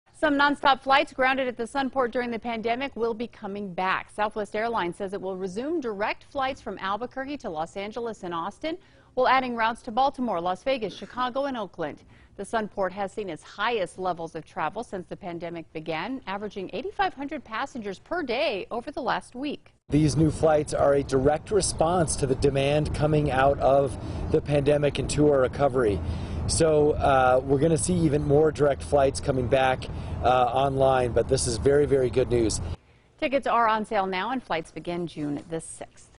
Southwest Airlines restores 2 non-stop flights from Sunport
Southwest Airlines restores two non-stop flights from the Albuquerque International Sunport and adds four others.